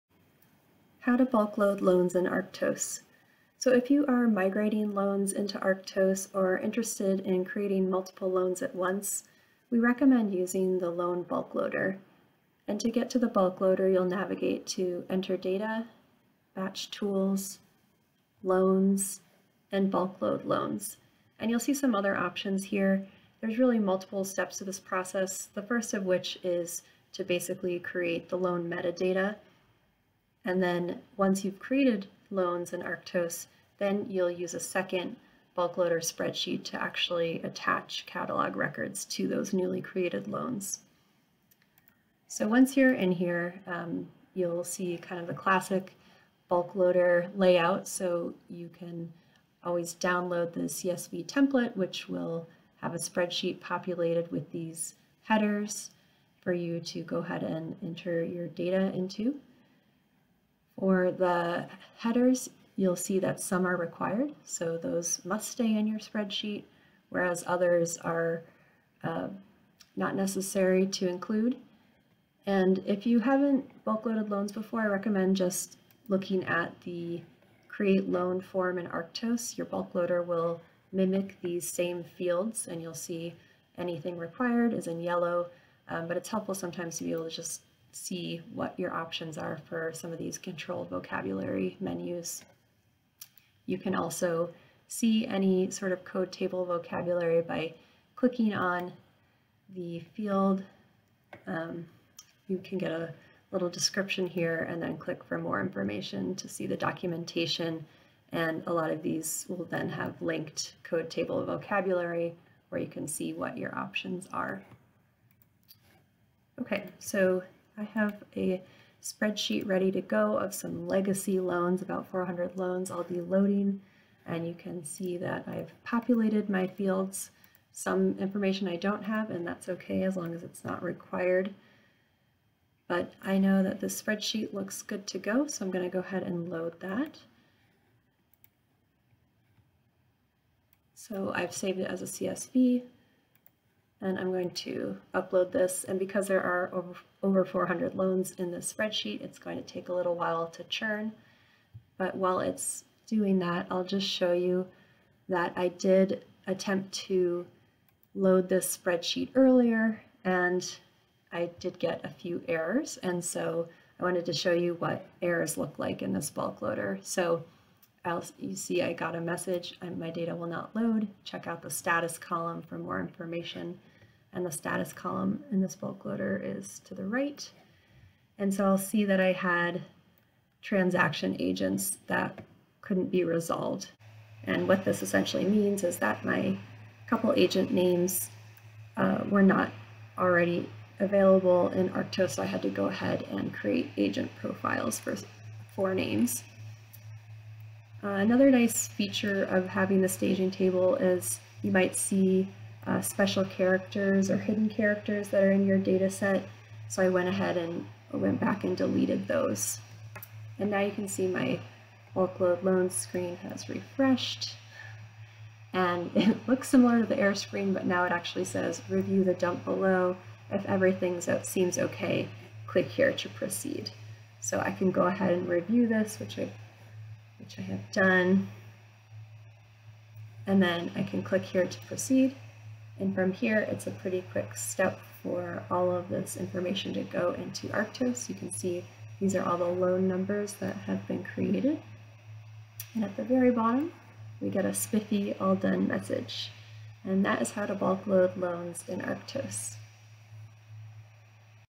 How to Bulkload Loans in Arctos
Learn how to bulkload loans in Arctos. This tutorial is the first of two bulkload steps for batch uploading loans (step 1: loan metadata; step 2 add loan items). for step 2.

Presenter: Emily Braker (Vertebrate Collections Manager, University of Colorado Museum of Natural History)